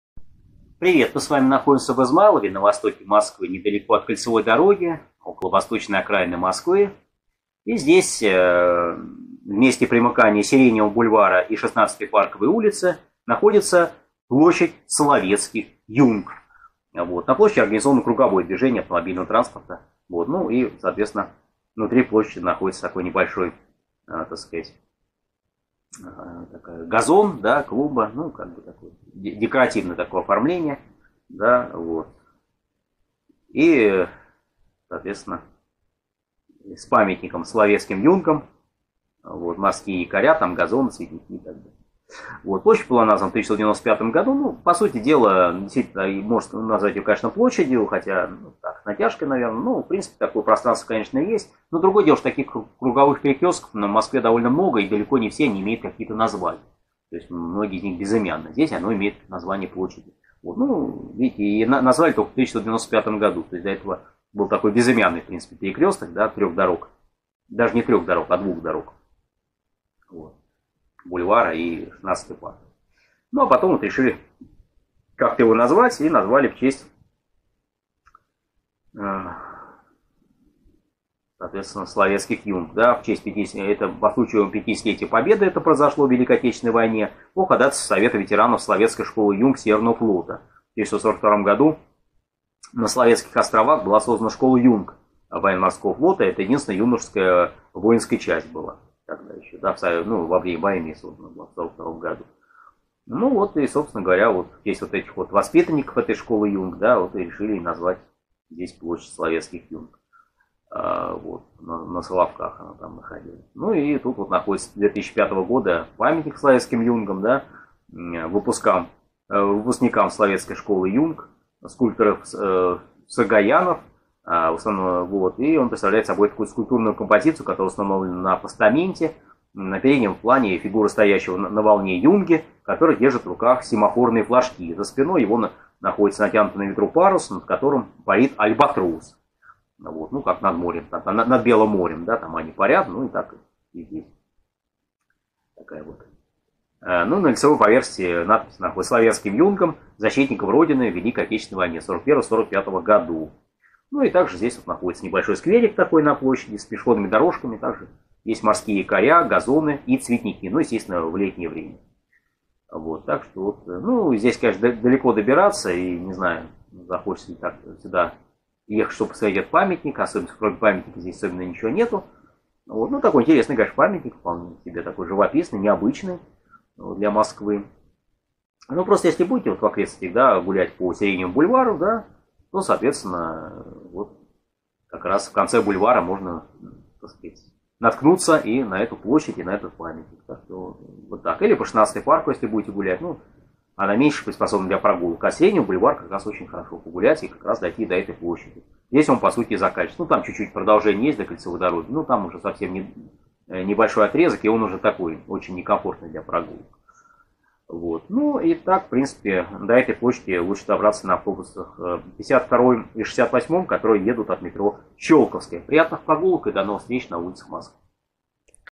Площадь Соловецких юнг
Рассказ про площадь Соловецких юнг в Измайлове. Автор – Андрей Плюснин.